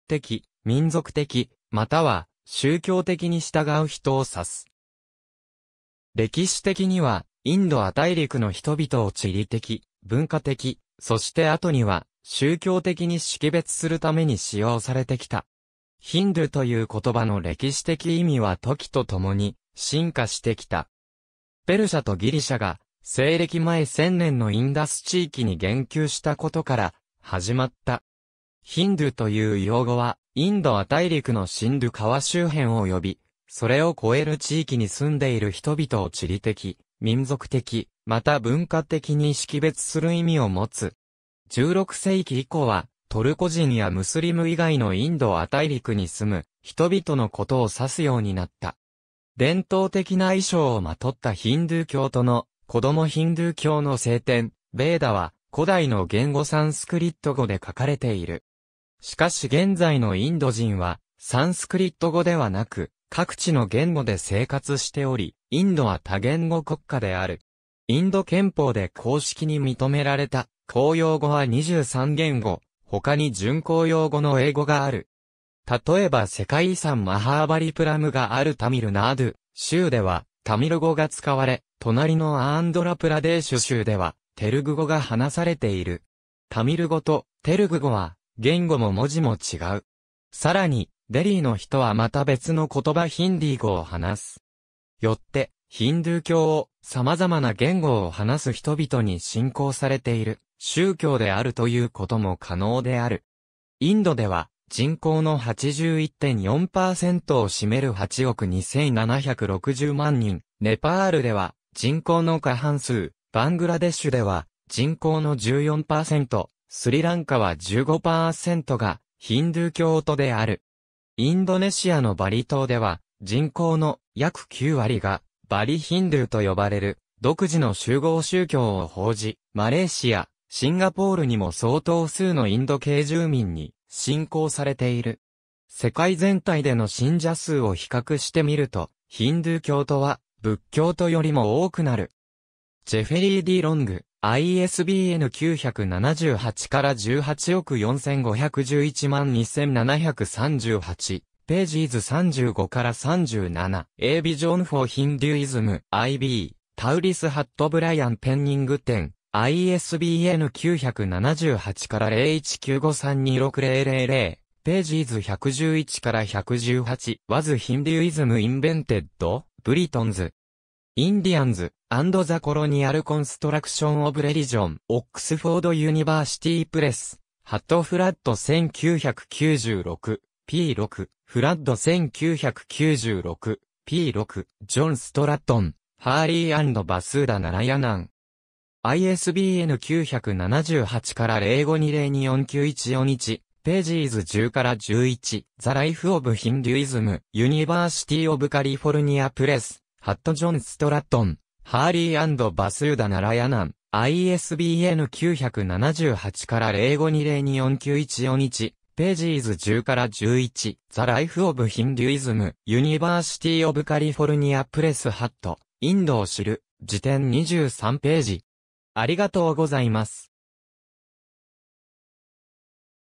ヒンドゥー教徒
ヒンドゥー教徒とは、ヒンドゥー教に関して文化的、民族的、または宗教的に従う人を指す。
歴史的には、インド亜大陸の人々を地理的、文化的、そして後には宗教的に識別するために使用されてきた。
ヒンドゥーという言葉の歴史的意味は時とともに進化してきた。
ペルシャとギリシャが西暦前1千年のインダス地域に言及したことから始まった。
ヒンドゥーと言う用語は、インド亜大陸のシンドゥ（インダス）川周辺及びそれを超える地域に住んでいる人々を地理的、民族的、また文化的に識別する意味を持つ。
16世紀以降は、トルコ人やムスリム以外のインド亜大陸に住む人々のことを指すようになった。
伝統的な衣装をまとったヒンドゥー教徒の子供 ヒンドゥー教の聖典「ヴェーダ」は古代の言語サンスクリット語で書かれている。
しかし現在のインド人はサンスクリット語ではなく、各地の言語で生活しており、インドは多言語国家である。
インド憲法で公式に認められた公用語は23言語、他に準公用語の英語がある。
例えば世界遺産マハーバリプラムがあるタミル・ナードゥ州ではタミル語が使われ、隣のアーンドラ・プラデーシュ州（数多くの遺跡があるハイデラバードを州都とする）ではテルグ語が話されている。
タミル語とテルグ語は言語も文字も違う。
更にデリーの人はまた別の言葉ヒンディー語を話す。
よってヒンドゥー教を「様々な言語を話す人々に信仰されている宗教である」ということも可能である。
インドでは人口の81.4%を占める8億2760万人、ネパールでは人口の過半数、バングラデシュでは人口の14%、スリランカは15%がヒンドゥー教徒である。
インドネシアのバリ島では人口の約9割がバリ・ヒンドゥーと呼ばれる独自の習合宗教を奉じ、マレーシア、シンガポールにも相当数のインド系住民に信仰されている。
世界全体での信者数を比較してみるとヒンドゥー教徒は仏教徒よりも多くなる。
^ Jeffery D. Long, ISBN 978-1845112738, pages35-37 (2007). A Vision for Hinduism. IB Tauris ^ Brian Pennington (2007), ISBN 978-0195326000, pages 111-118. Was Hinduism Invented?Britons,
Indians, and the Colonial Construction of Religion. Oxford University Press ^ Flood 1996, p. 6.. Flood 1996, p. 6. ^ John Stratton Hawley and Vasudha Narayanan (2006),
ISBN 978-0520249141, pages 10-11. The Life of Hinduism. University of California Press ^ John Stratton Hawley and Vasudha Narayanan (2006), ISBN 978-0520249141, pages 10-11. The Life of Hinduism. University of California Press ^ 『インドを知る辞典』 23頁。